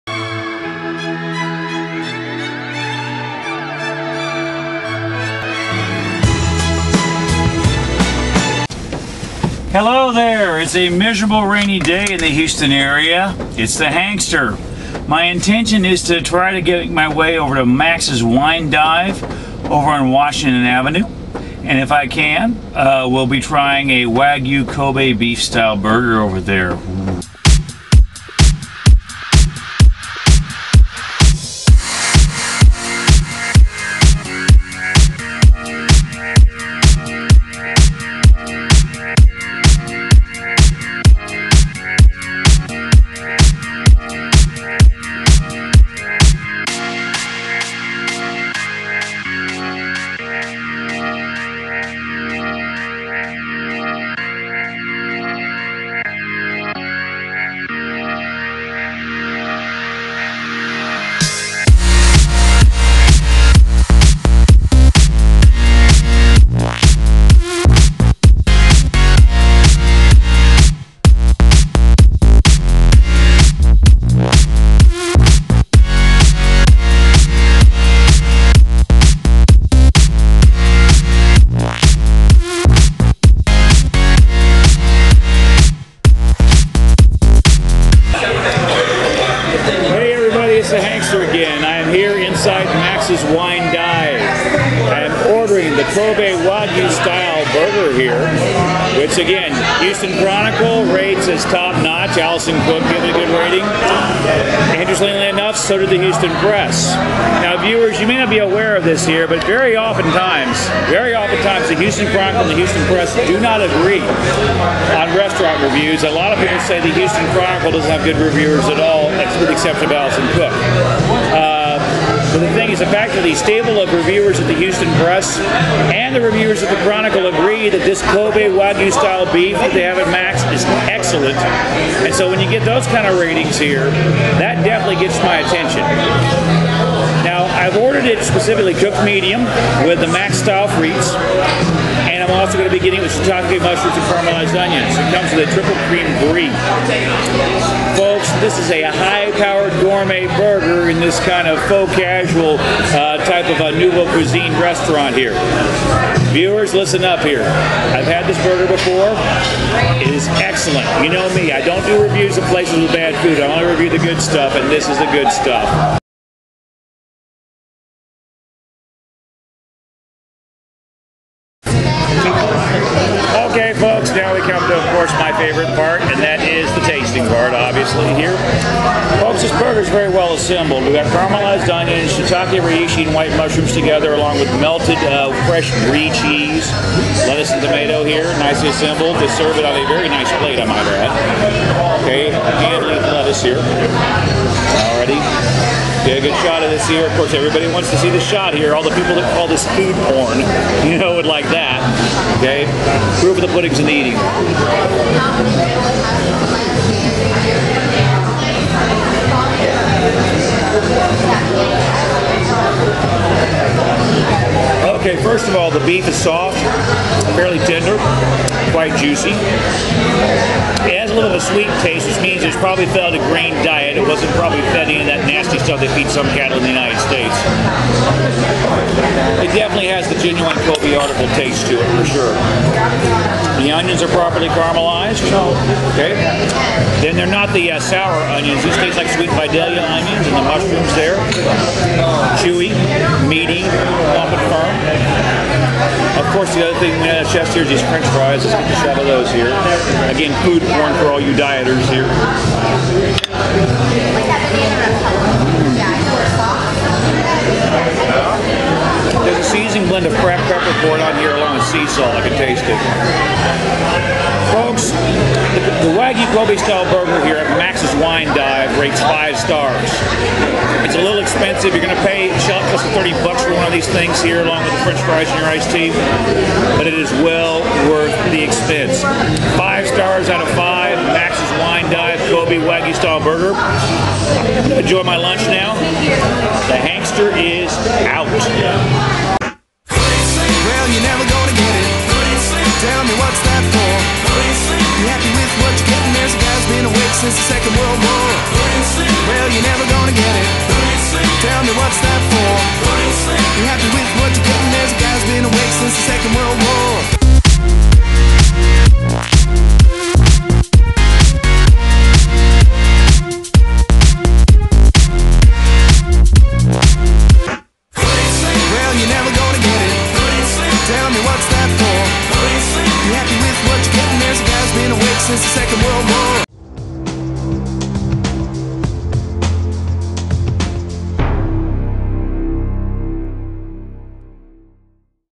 MaxswineDiveReview.flv
I went to Max's Wine Dive on a rainy Friday in Houston to review their Kobe Beef Burger.  It was WORTH the drive to eat it!